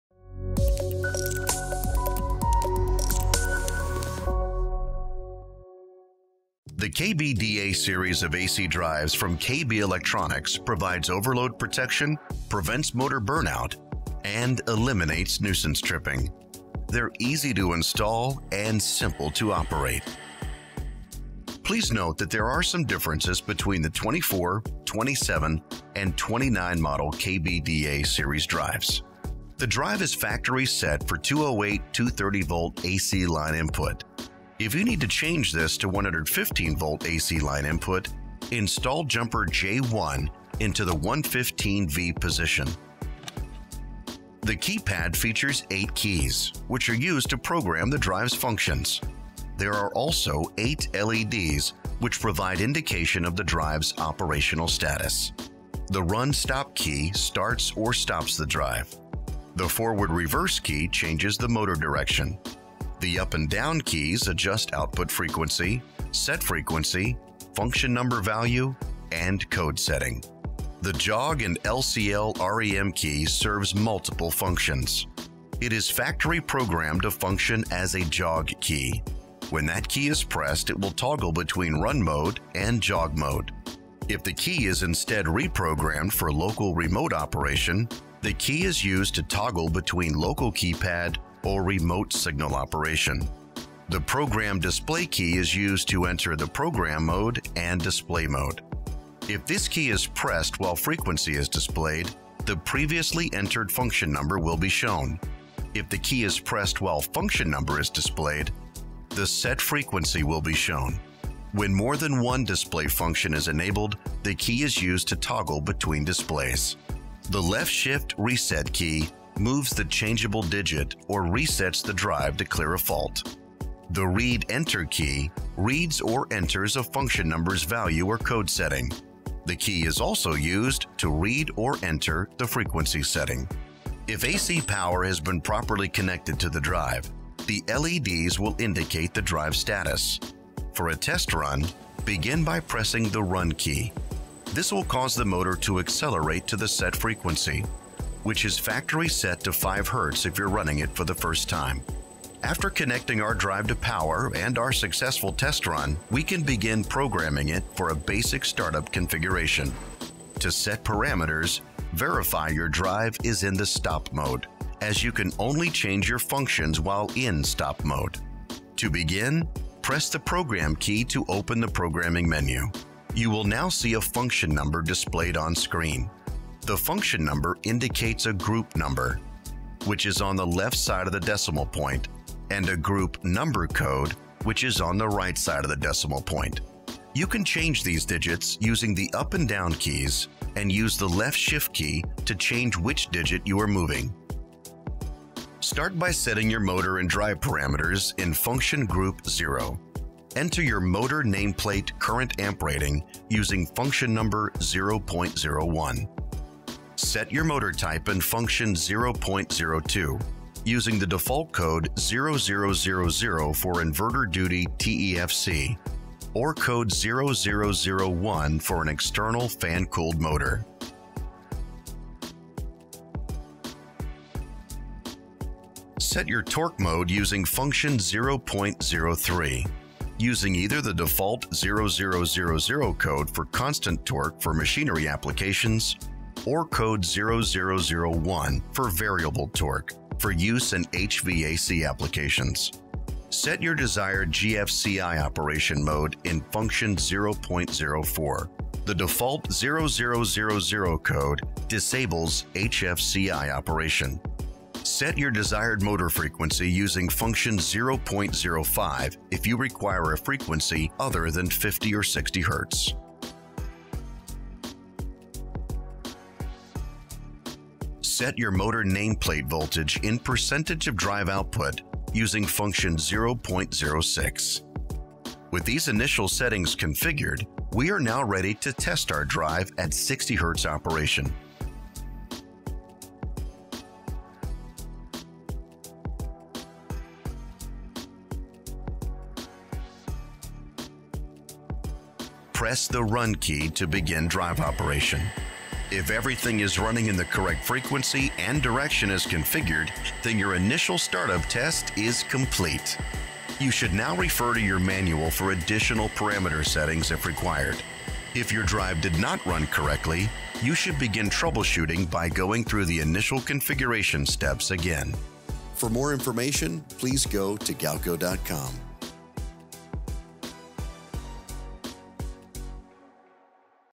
Basic Startup Process on KB Electronic's KBDA AC Drive
The KBDA series of AC Drives provides overload protection, prevents motor burnout and eliminates nuisance tripping. They're easy to install, energy saving, and simple to operate. The KBDA drive can be used indoors or outdoors, with no holes to drill, and no switches to install. They're customizable, which can include pre‑calibrating or programming of a stock control, adding a custom label or branding, custom software, PLC functions or designing a new control.